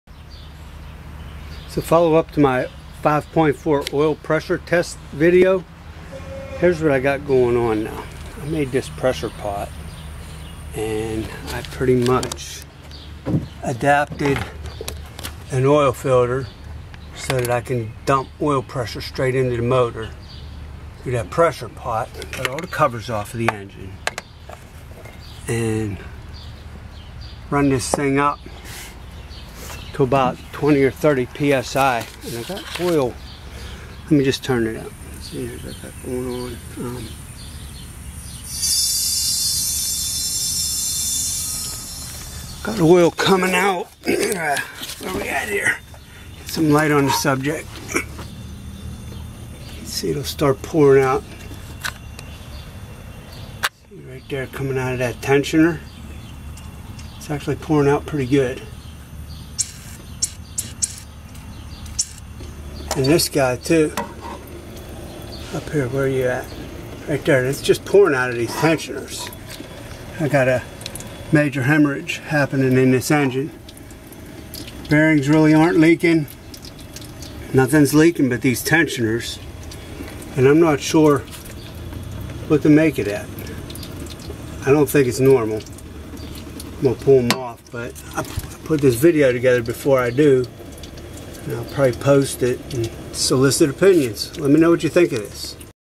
Ford 5.4L Internal Oil Pressure Leak Diagnosis
My technique used to determine erratic oil pressure problem in Ford 5.4L. I ended up dressing the front of the motor (tensioners, chains, guides, phasers, HV oil pump) and my original problem shown in my other video is solved.  I feel I could have probably been cheap and just hung tensioners, but I plan on keeping the vehicle long term so the correct fix was applied.  I'm thinking the leak had the oil pressure right at the edge of failure and when the VCT system was applied it drew too much volume away from the already compromised system.  Additional bonus is that this diagnosis helped to ensure there weren't other problems before investing $850 in timing related parts.